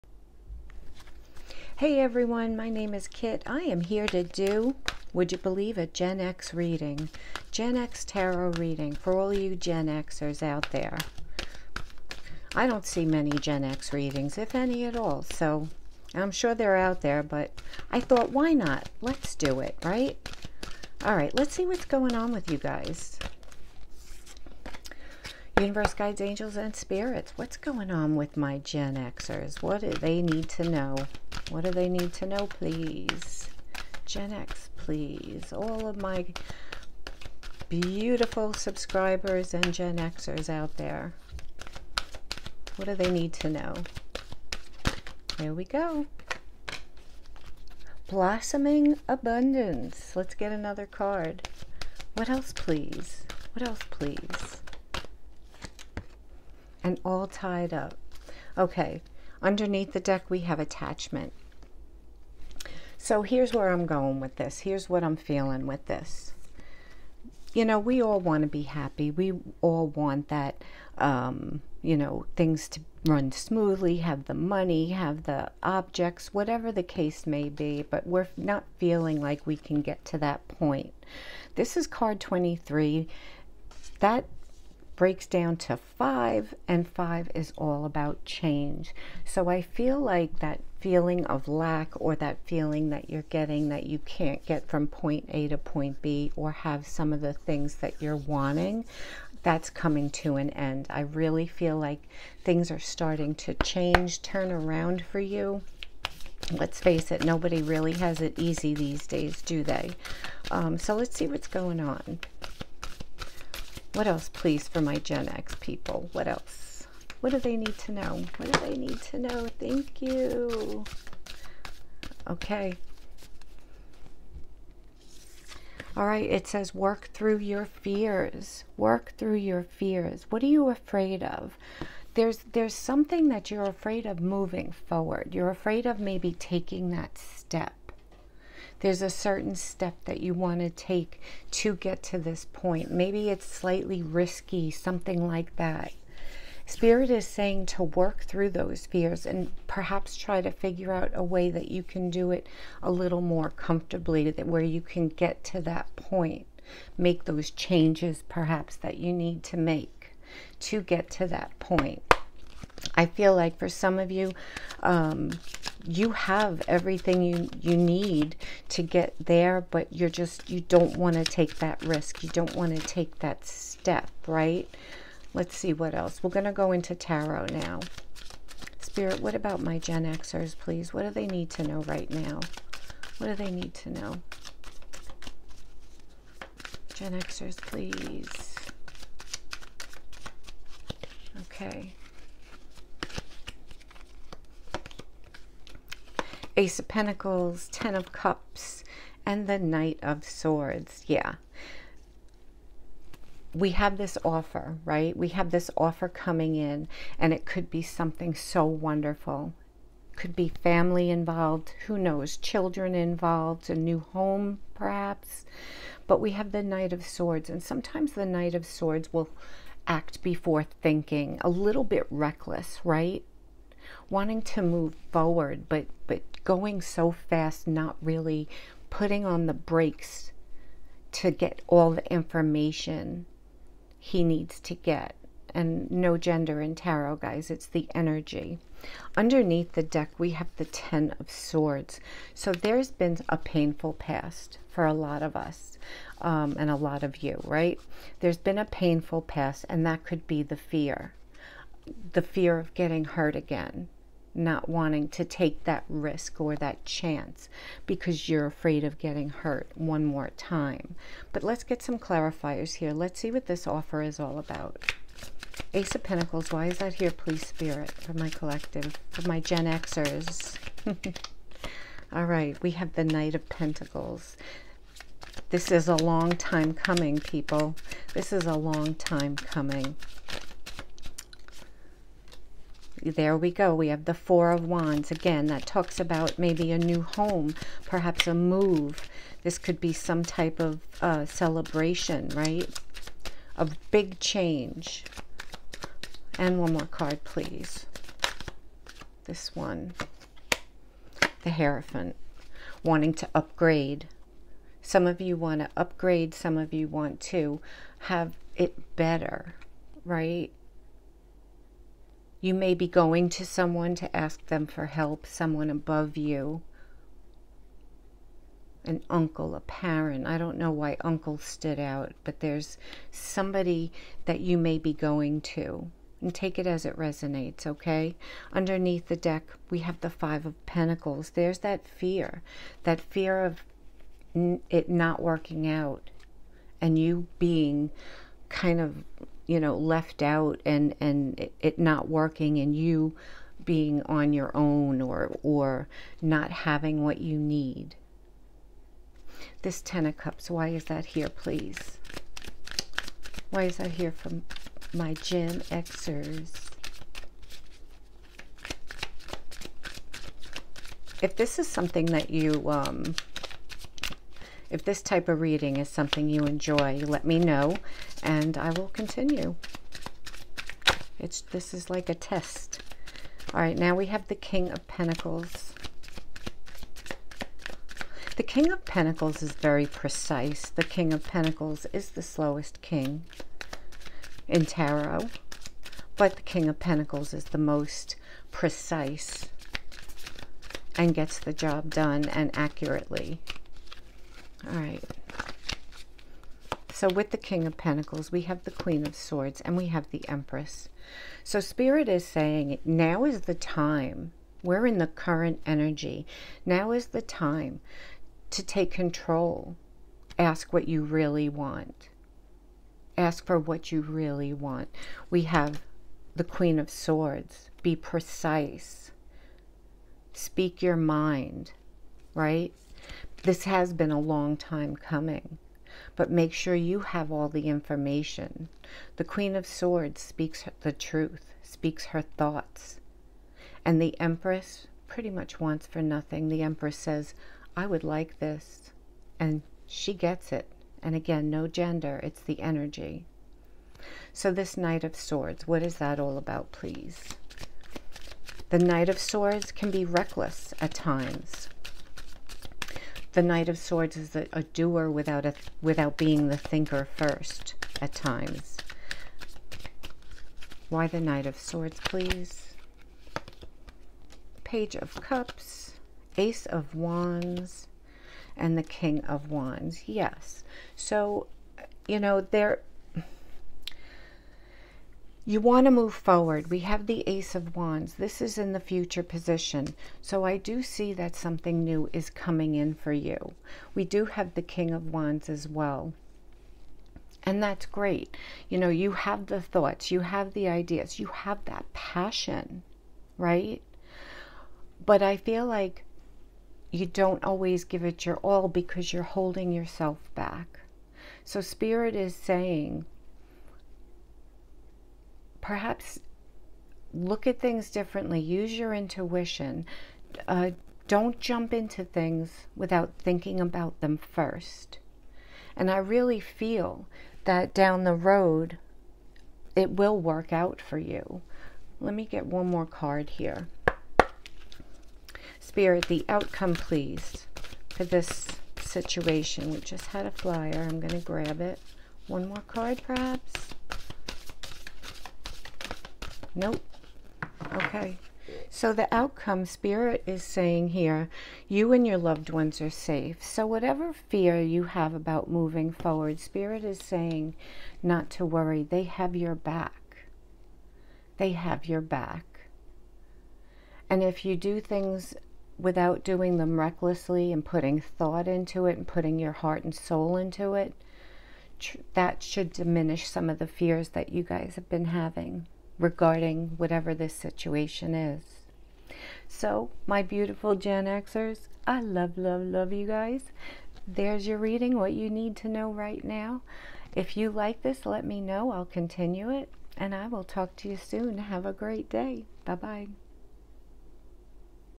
GEN X Only! What You Need To Know RIGHT NOW!
This is a general timeless tarot reading for Gen X people only! What do my Gen Xers need to know right now? If this is something you’d like to see in the future or if you’d like to see more of this type of reading the thumbs up and let me know in the comments!